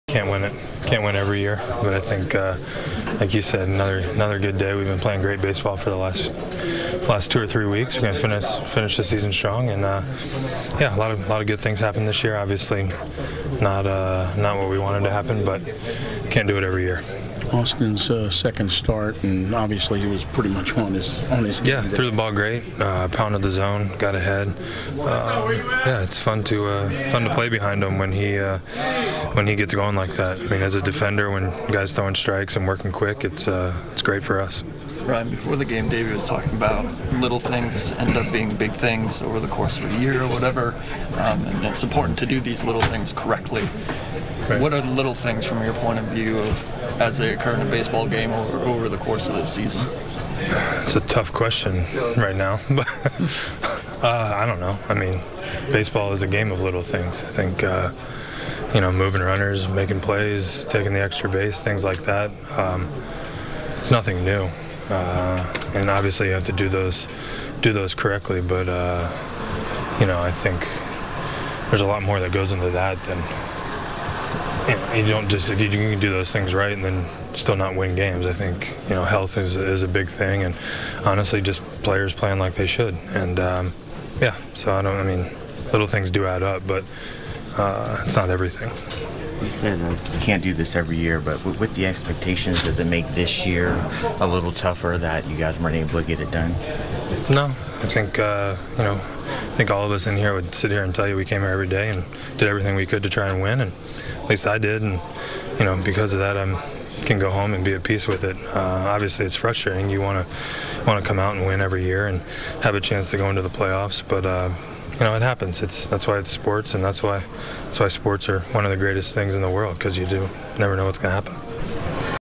Ryan Zimmerman on Nationals 2018 season
Ryan Zimmerman talks about the Nats being eliminated from playoff contention. More